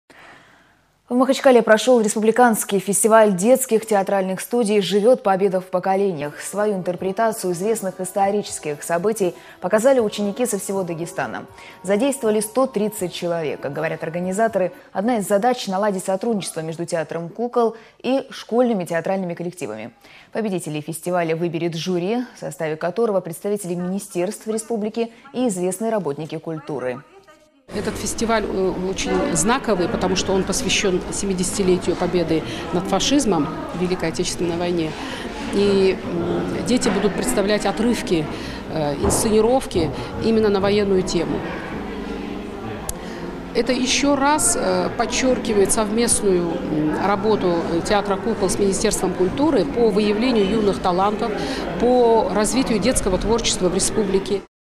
"Живёт Победа в поколеньях"
В Махачкале прошел республиканский фестиваль детских театральных студий «Живёт Победа в поколеньях».